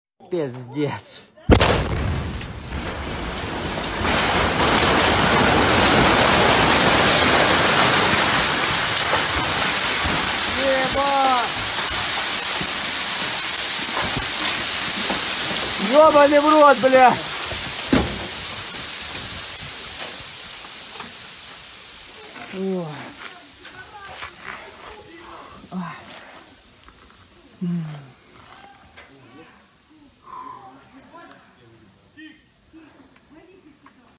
Explosion Goes Off Behind Guy in Ukraine || ViralHog
Occurred on September 13, 2022 / Mykolaivka, Donetsk Region, Ukraine

Arrival Mykolaivka (Donetsk region), no one was hurt.